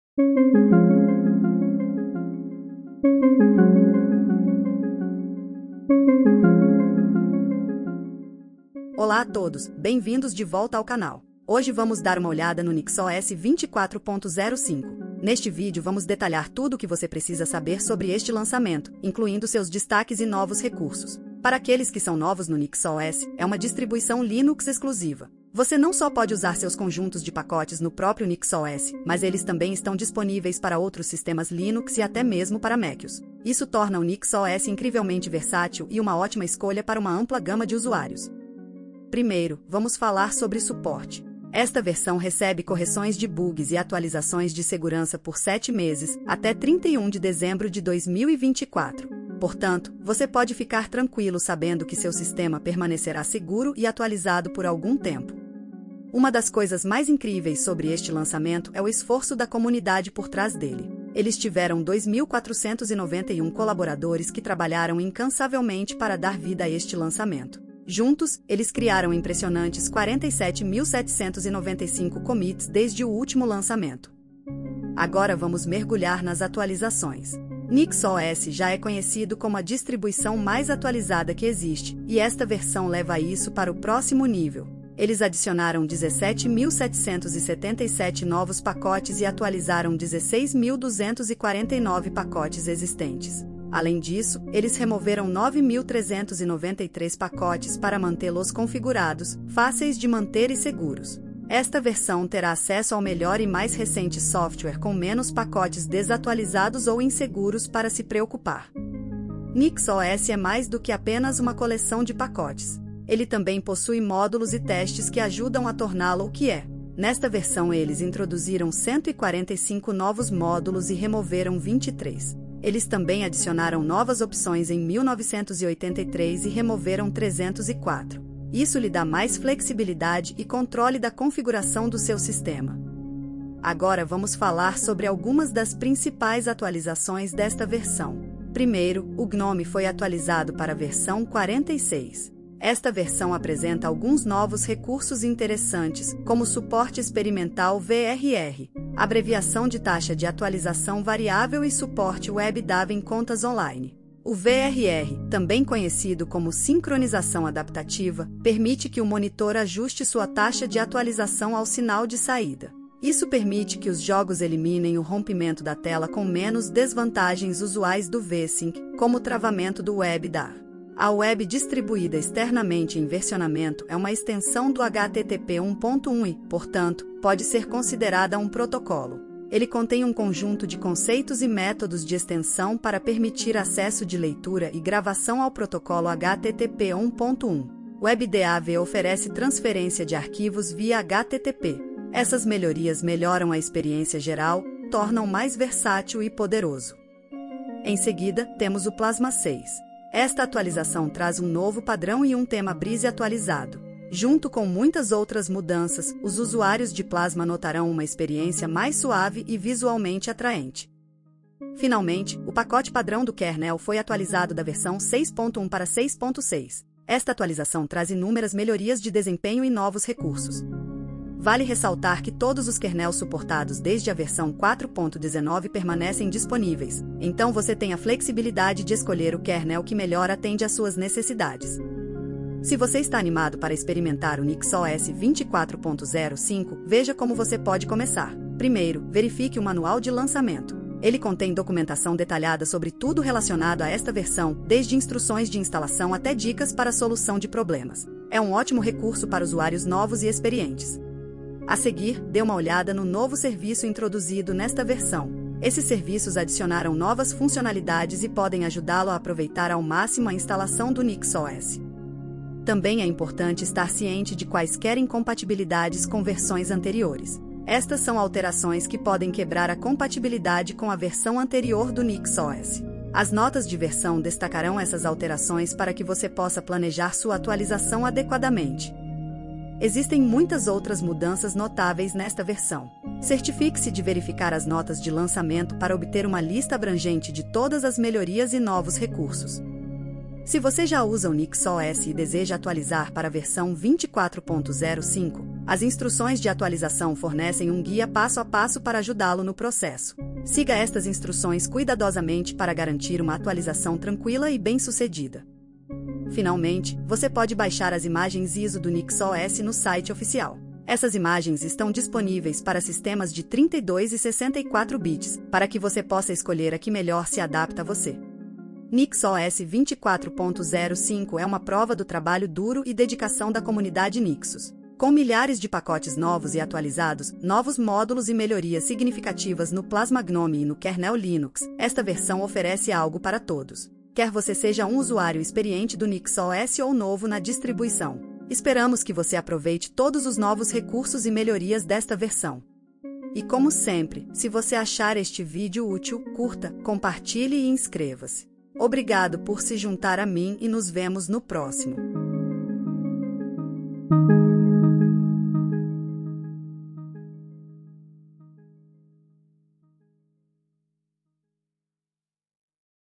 NixOS 24.05 chega com Kernel Linux 6.6 LTS, GNOME 46, KDE Plasma 6 e Lomiri
A distro independente NixOS 24.05 já está disponível para download com os ambientes de desktop GNOME 46 e KDE Plasma 6, kernel Linux 6.6 LTS.